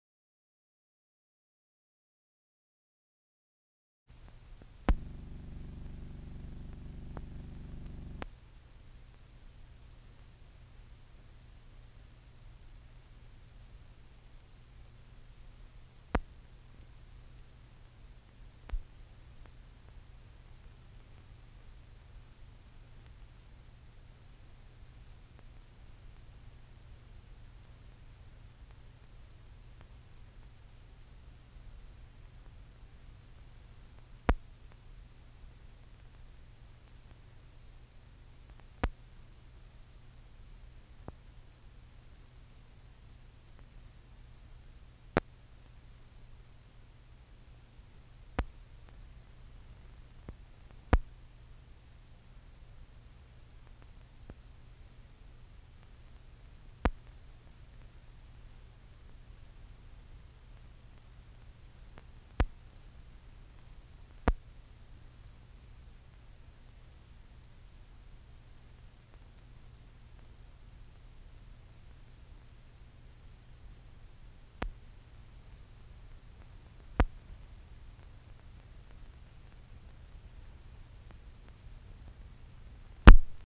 Basketball match won by Long Island University's Blackbirds over Valparaiso at Ma...HD Stock Footage
Basketball match won by Long Island University's Blackbirds over Valparaiso at Madison Square Garden in New York.

Basketballs match between Long Island University's Blackbirds and Valparaiso at Madison Square Garden in New York. Match in progress. Spectators watch. Crowd applauds. The Blackbirds win. Location: New York United States. Date: December 6, 1945.

57,000+ broadcast-quality historic clips for immediate download.